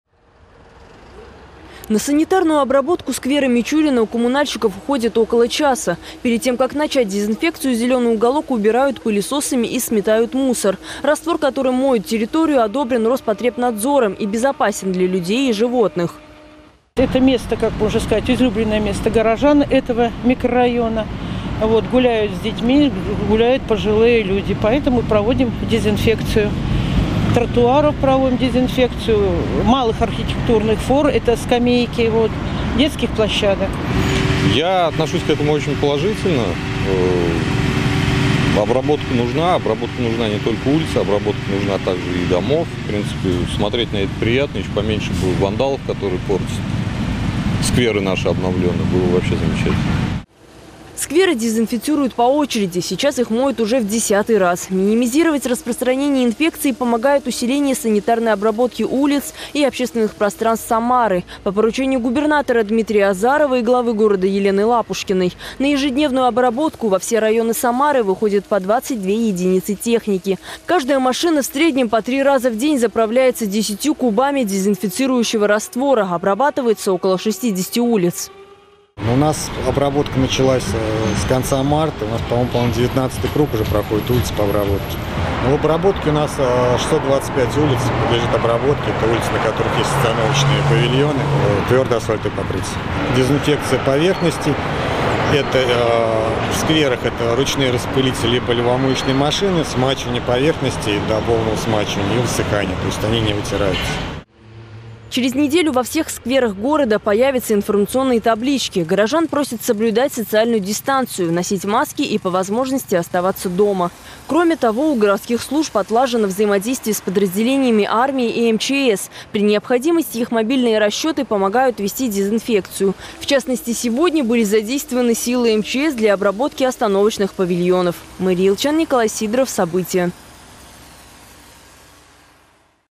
В Самаре продолжают обрабатывать общественные пространства
За сутки в Самарской области зарегистрировали 110 новых случаев заражения коронавирусной инфекцией. Общее число заболевших COVID-19 в регионе с начала пандемии — 11 551. Чтобы не допустить распространение инфекции, в Самаре продолжают обрабатывать общественные пространства. Обеззараживают даже проезжую часть. В этом убедилась Мери Елчян.